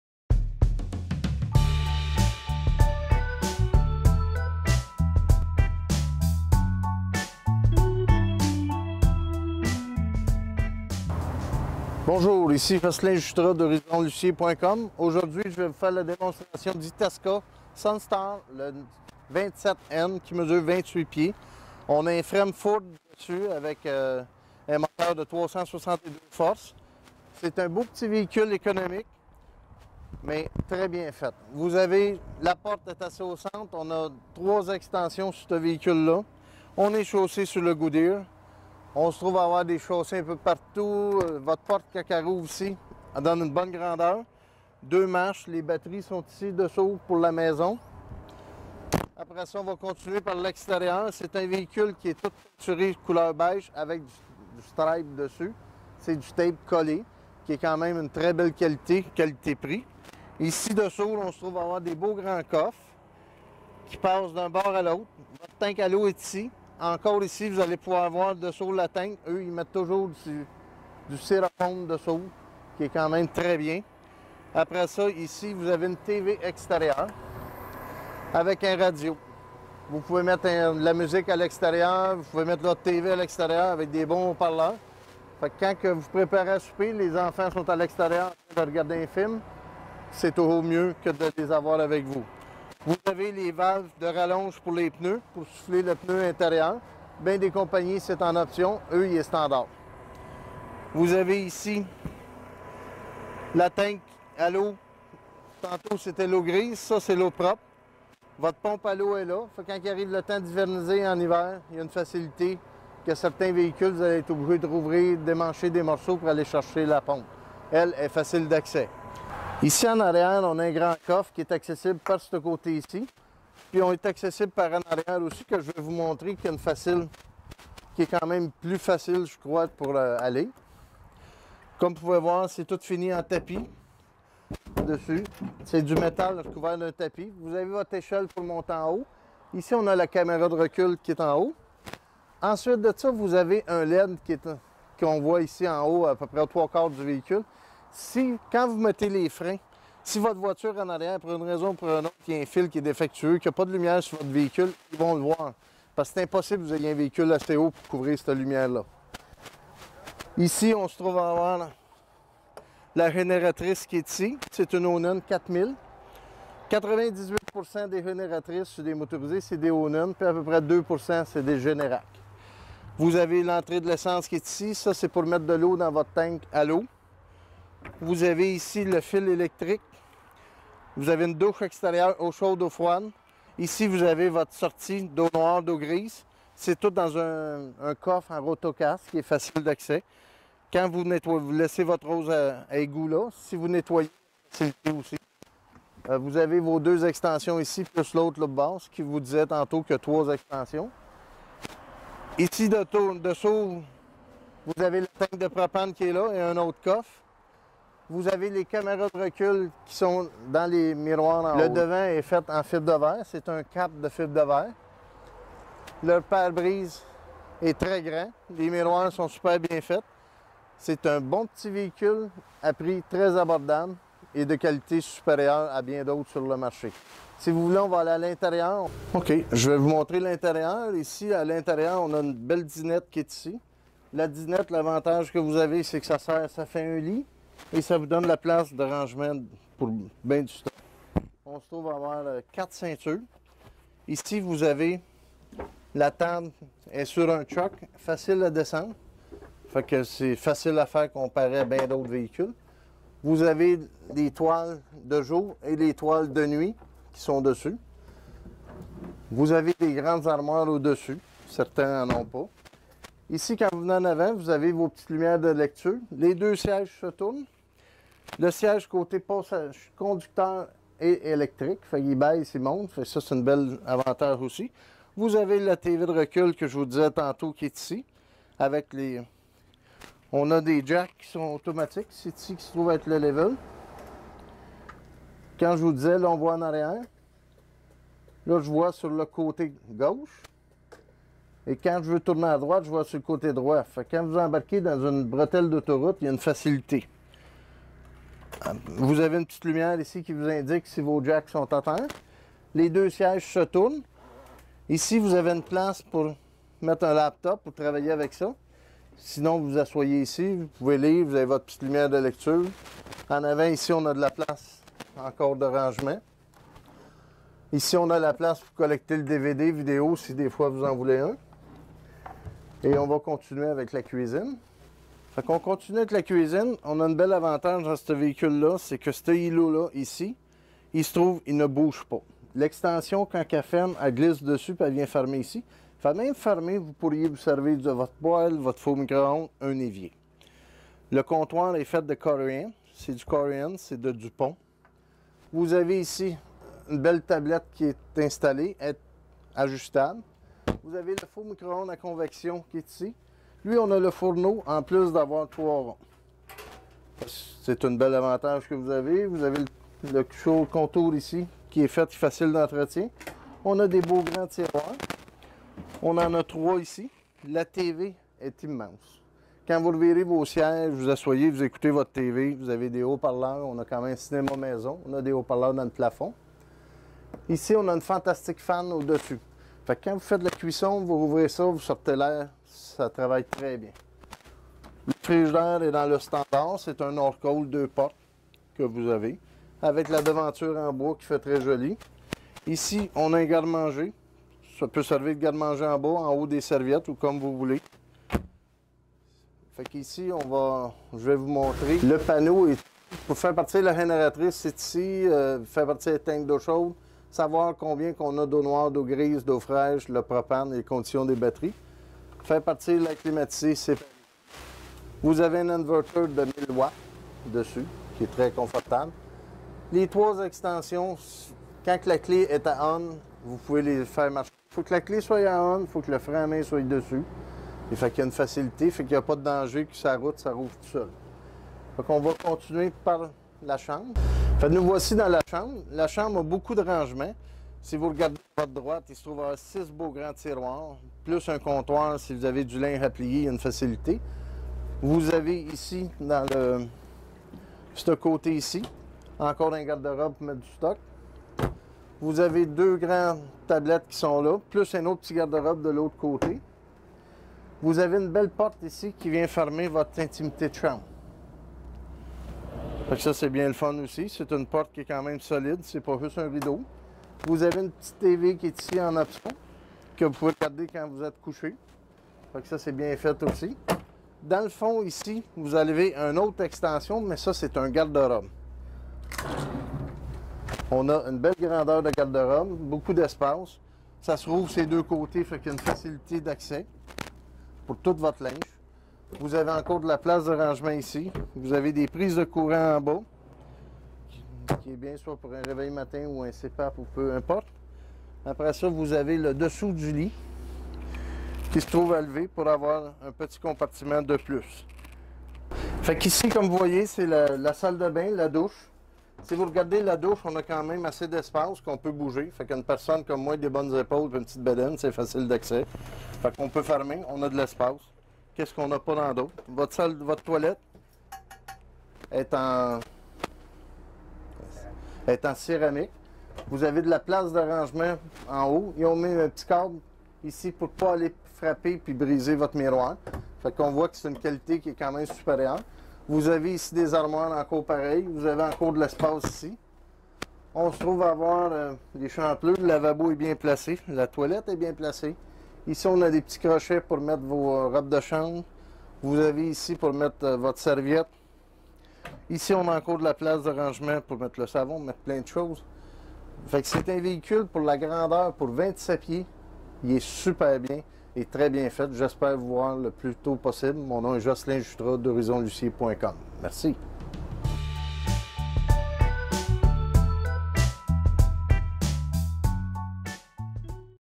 Motorisé classe A Itasca sunstar 27N video
Présentation vidéo du motorisé classe A Itasca Sunstar 27N offert chez Horizon Lussier et VR Dépôt.